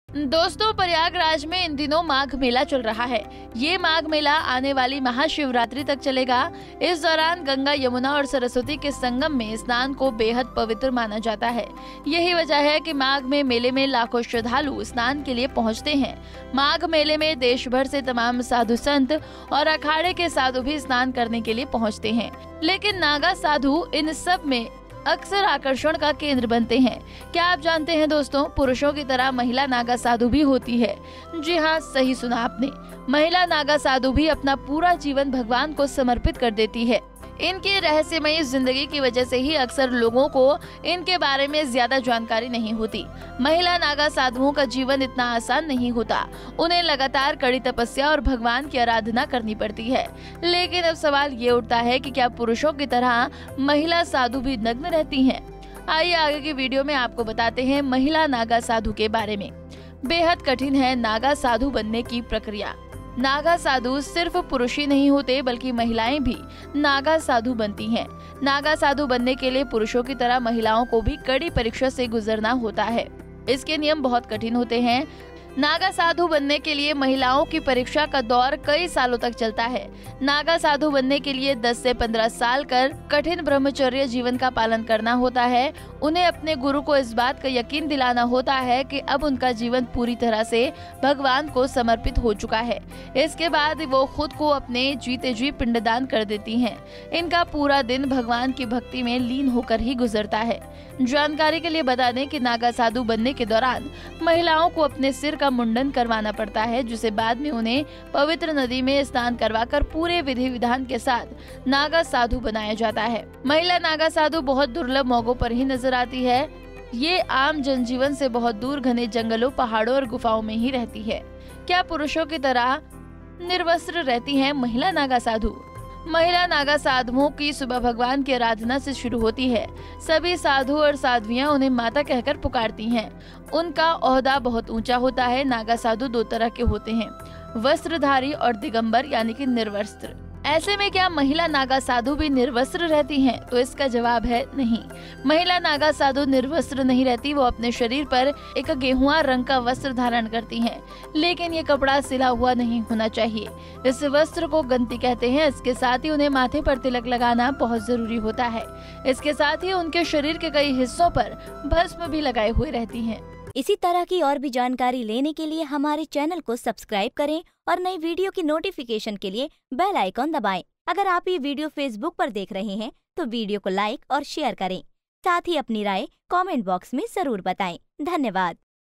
क्या बिना कपड़ों के रहती हैं महिला नागा साधु ? जानिए, पूरा सच | Mahila Naga Sadhu
header
पुरुषों की तरह महिलाएं भी होती हैं नागा साधु
महिला नागा साधु पहनती हैं एक गेरुआ रंग का वस्त्र
महिला नागा साधुओं का जीवन आसान नहीं होता है
महिलाओं को अपने सिर का मुंडन करवाना पड़ता है